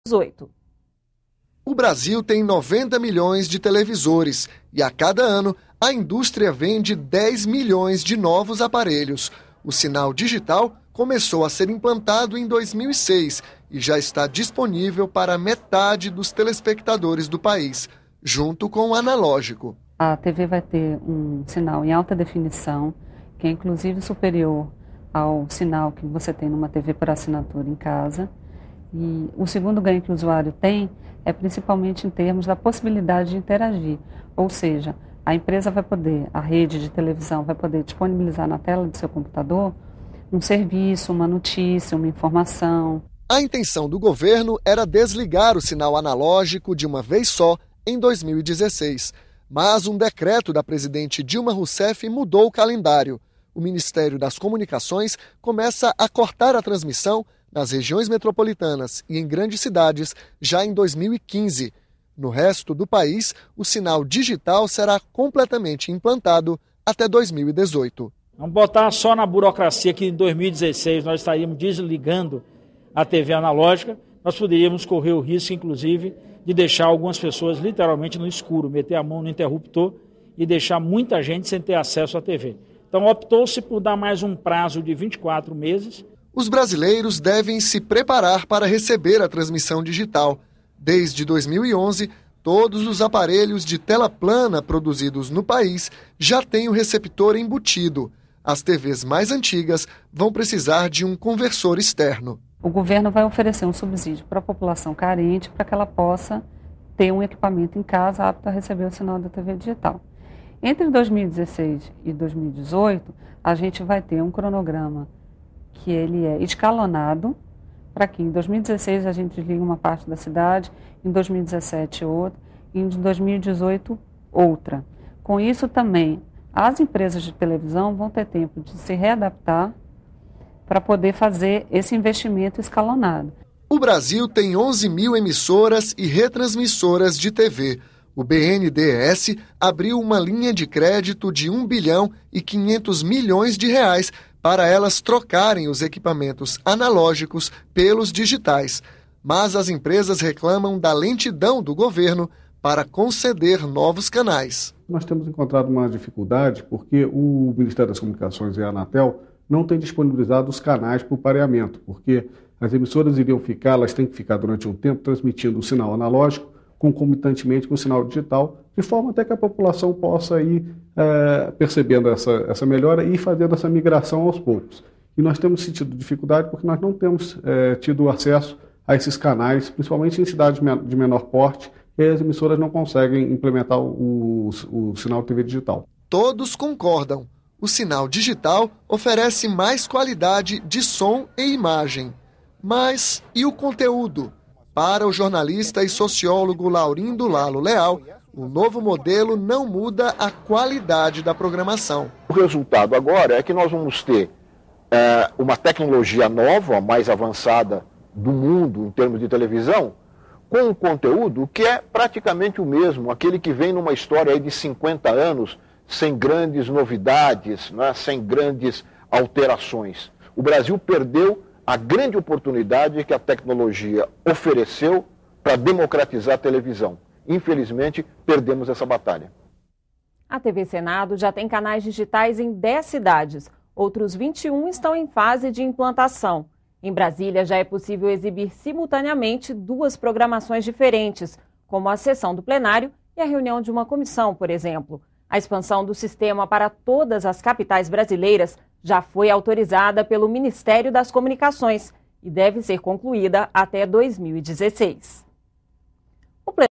Jornalismo - Transição do sinal analógico de TV para o digital será feita gradualmente
O governo adiou o fim das transmissões analógicas de 2016 para 2018